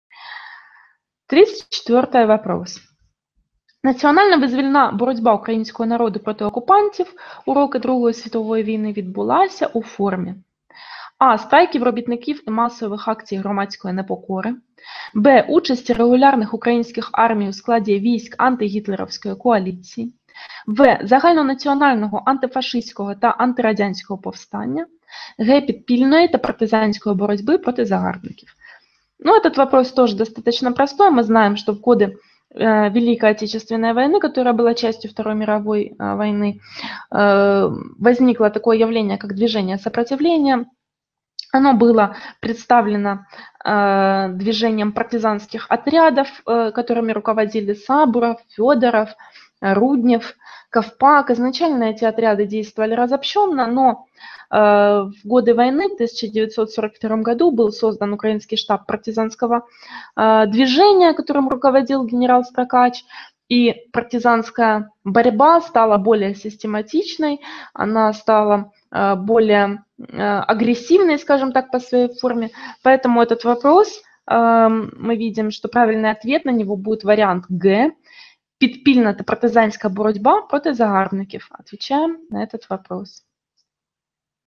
Решение ЗНО по истории Украины 2014 задание 33
Для якого року були характерними події, описані в уривку з історичного джерела: «У селі Копилів Сокальського повіту Львівського воєводства знищене приміщення читальні, подерто бібліотечні книжки і портрети Шевченка і Франка. Після знищення читальні установлено людей перед читальнею і змушено їх кричати: "Хай живе Пілсудський!"...»?